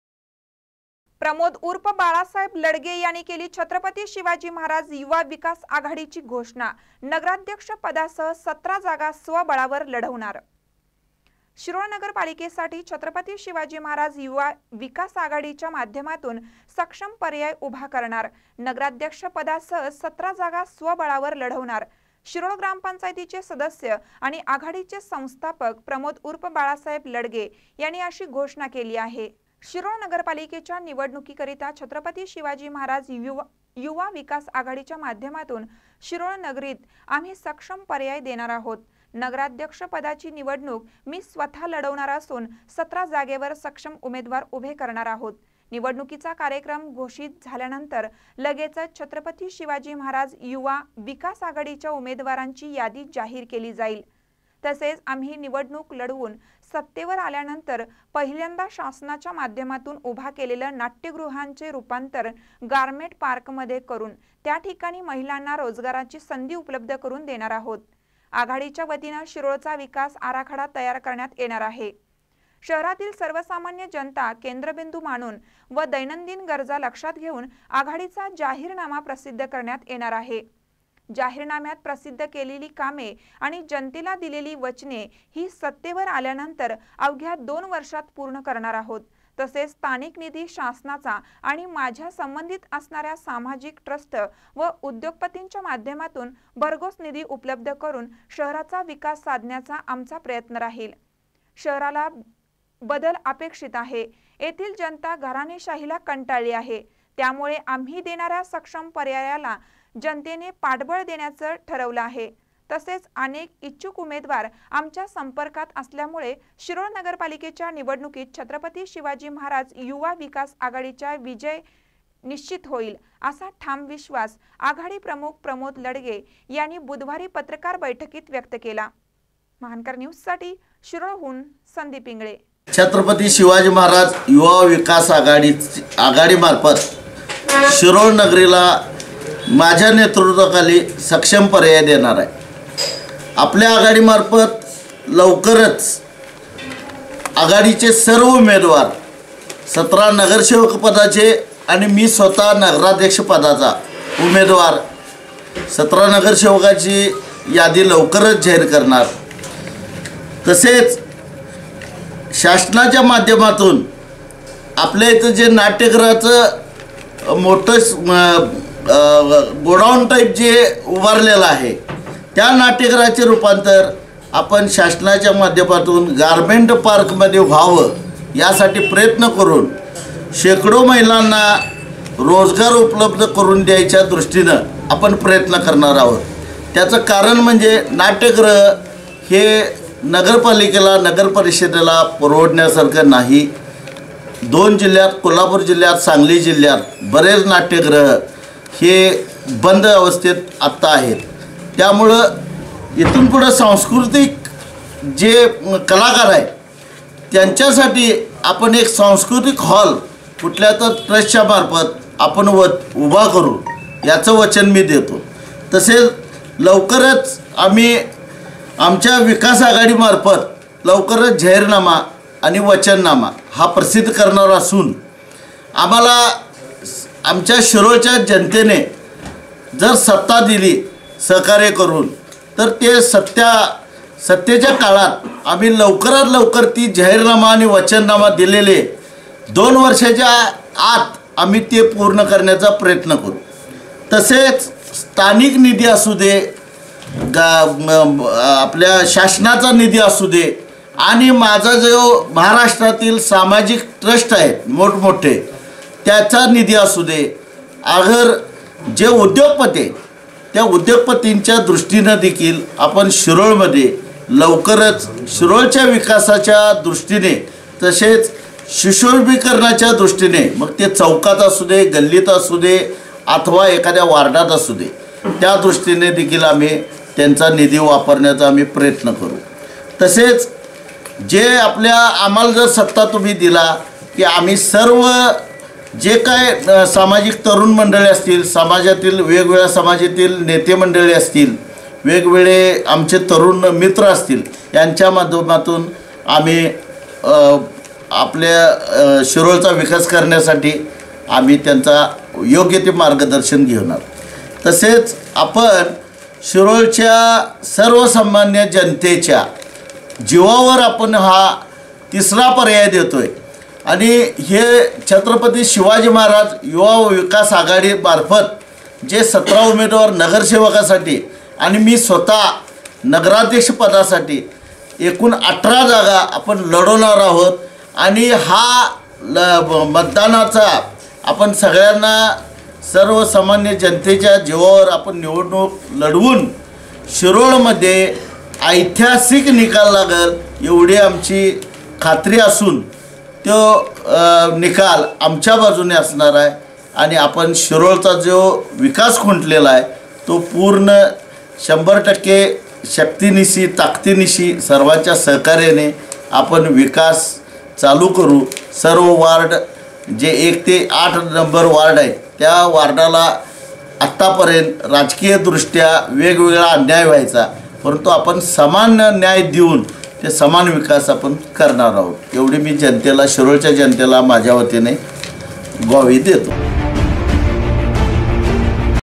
प्रमोद उर्फ बाळासाहेब लडगे यांनी केली छत्रपती शिवाजी महाराज युवा विकास आघाडीची घोषणा
प्रमोद उर्फ बाळासाहेब लडगे यांनी केली छत्रपती शिवाजी महाराज युवा विकास आघाडीची घोषणा ; नगराध्यक्षपदासह १७ जागा स्वबळावर लढवणा र